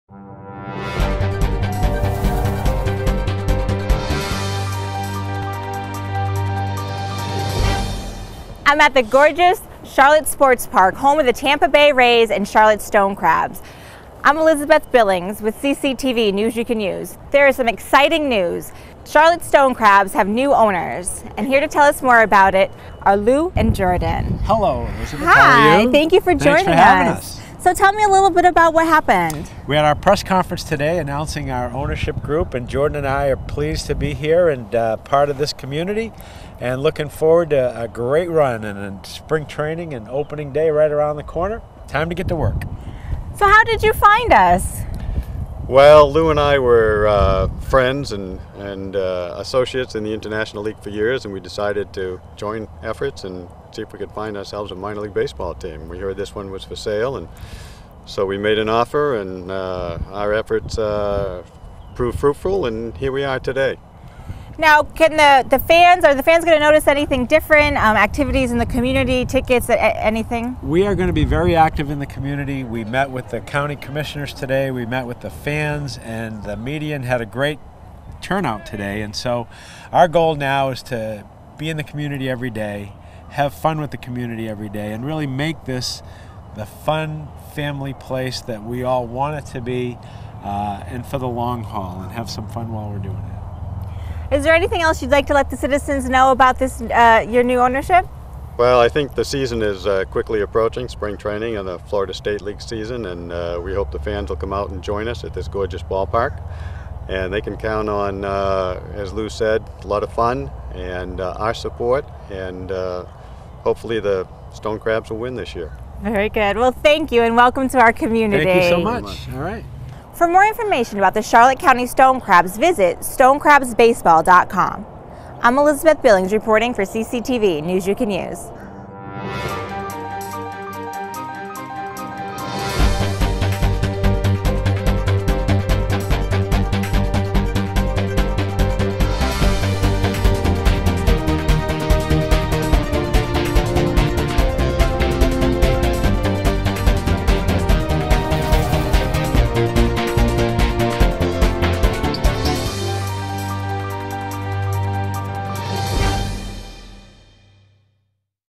NYCU New Stone Crabs Owners
A video introducing the new owners of the Charlotte Stone Crabs to the community.